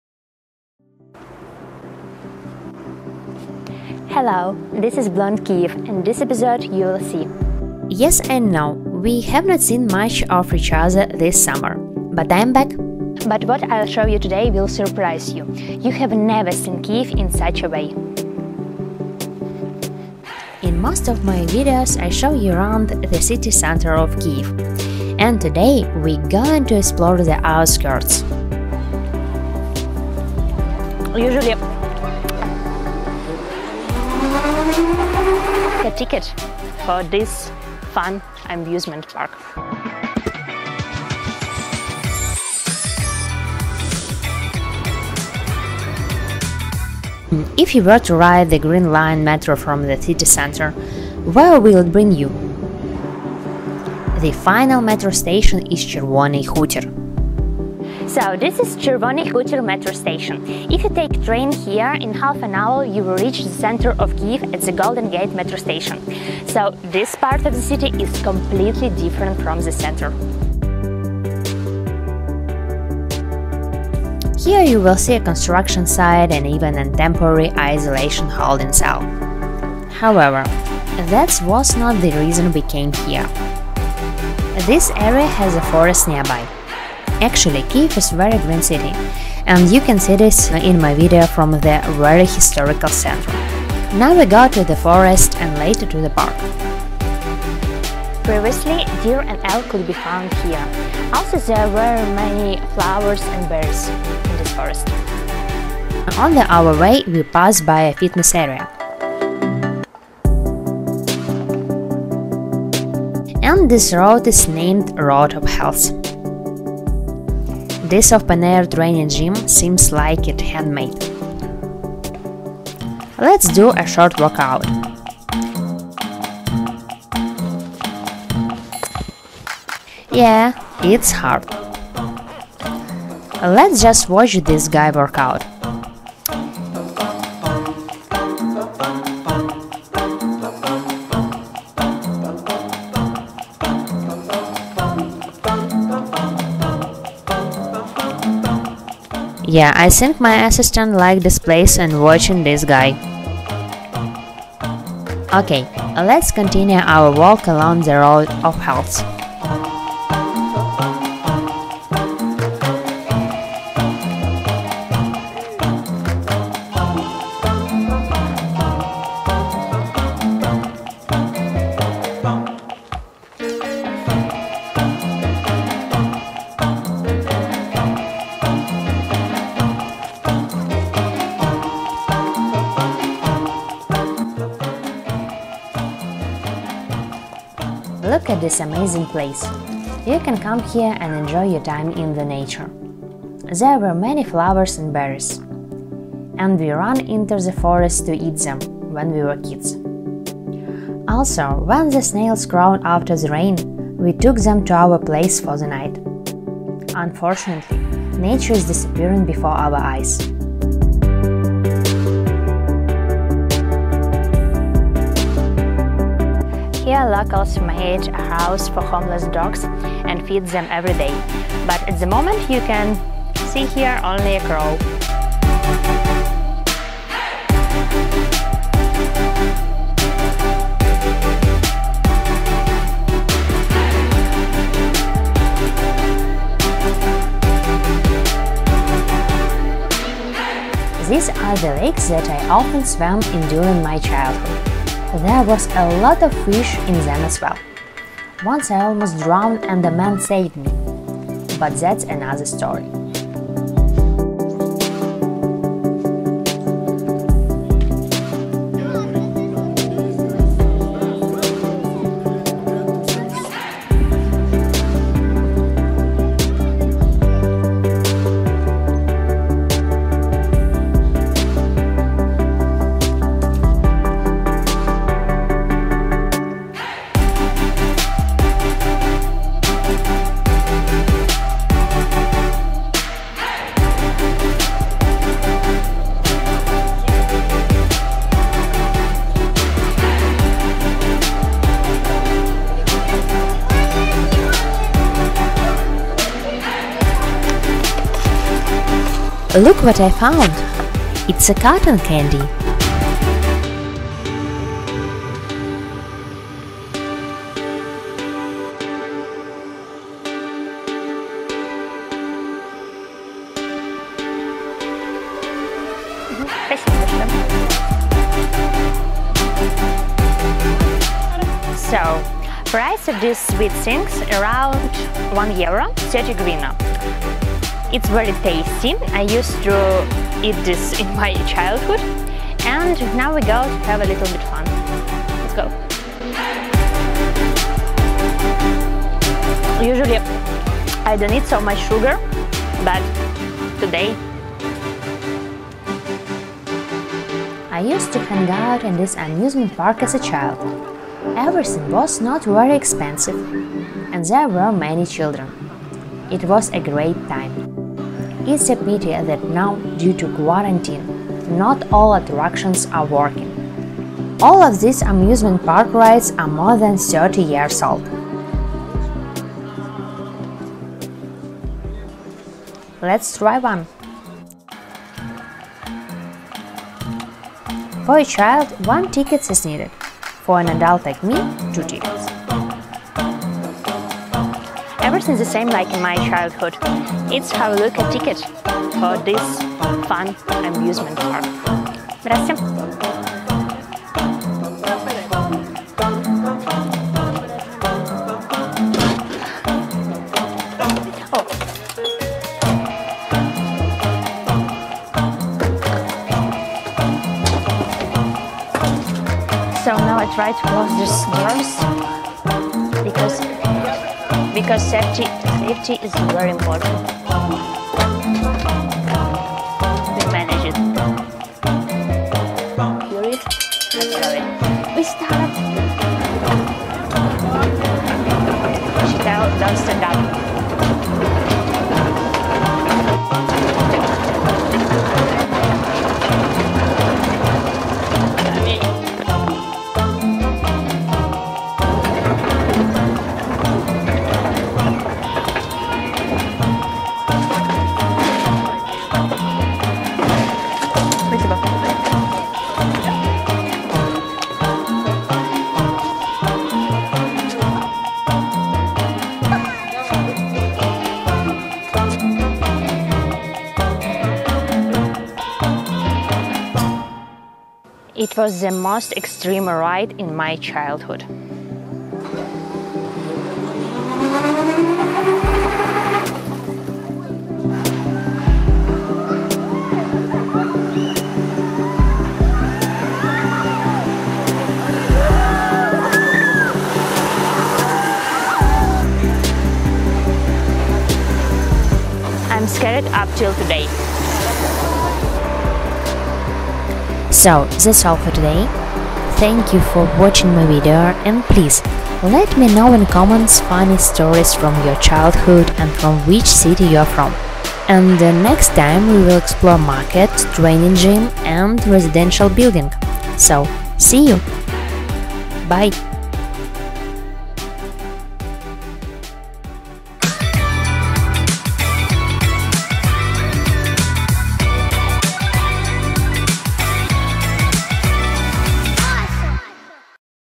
Beneath Kyiv : Soviet Amusement Park [back to USSR] Kyiv outskirts
Hello. This is BlondeKyiv.
We have not seen much of each other this summer. It was a very busy season, and I took a break from YouTube.
In most of my videos, I show you around the city center of Kyiv. Today, we are going to explore the outskirts.

00:46 The final metro station is Chervonyi Khutir
01:30 Forest nearby
01:48Fitness area and handmade training gym
02:00 Lakes forest park
03:00 Cotton candy
04:00 Amusement park and amusement rides

Let me know in comments funny stories from your childhood and from which city you are from :)